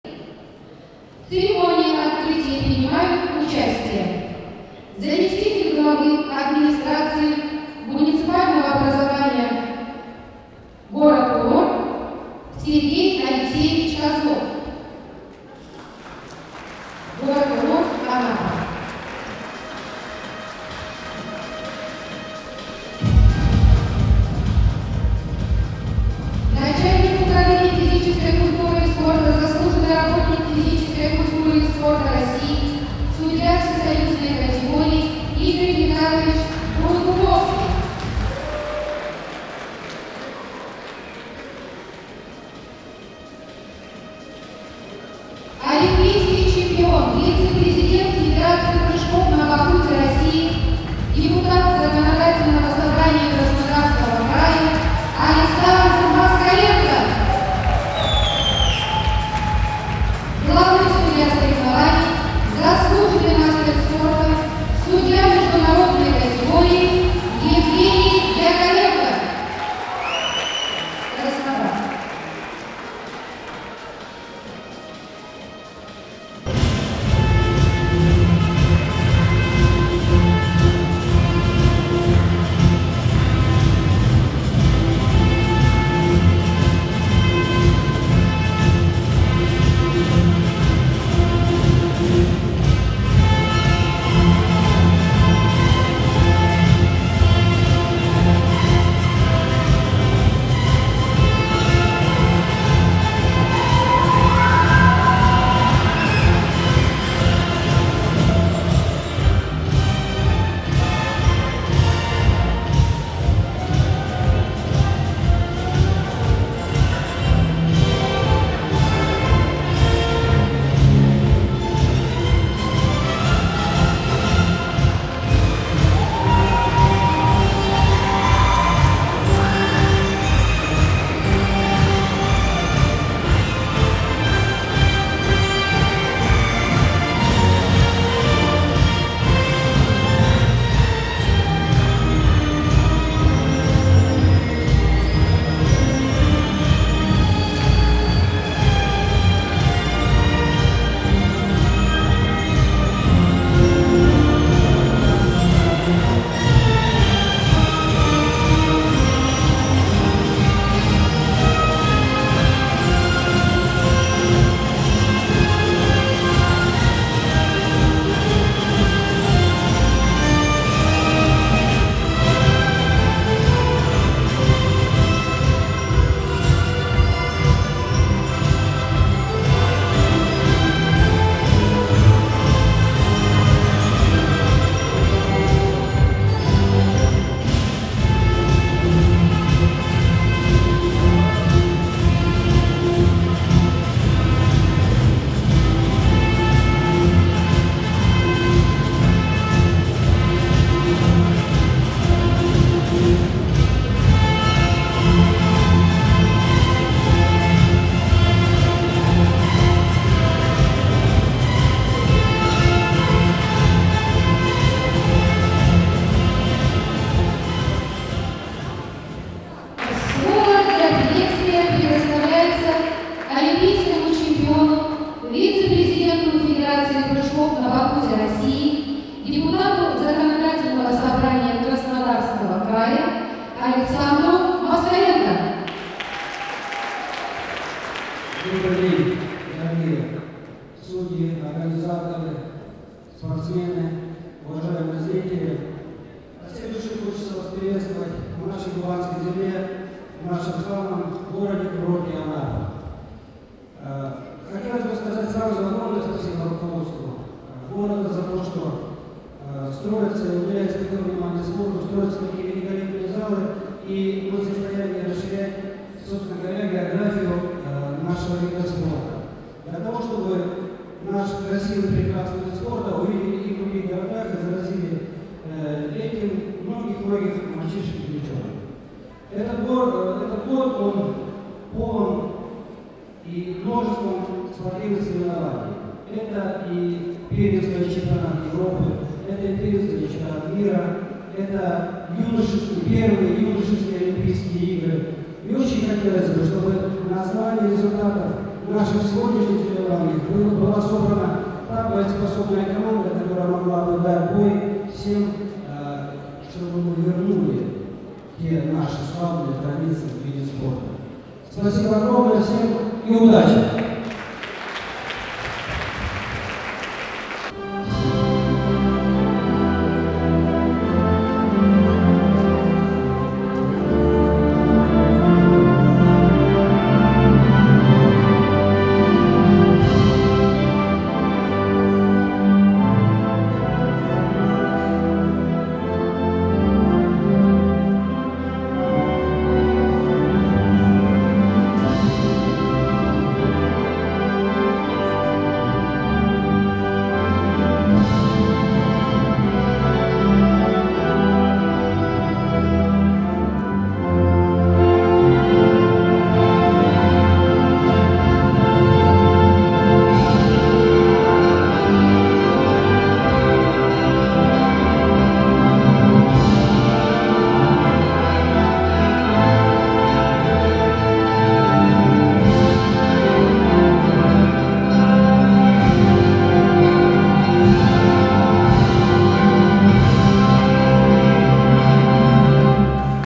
otrkrytie
Церемония открытия командного Чемпионата России по прыжкам на батуте, АКД и ДМТ, пос. Витязево, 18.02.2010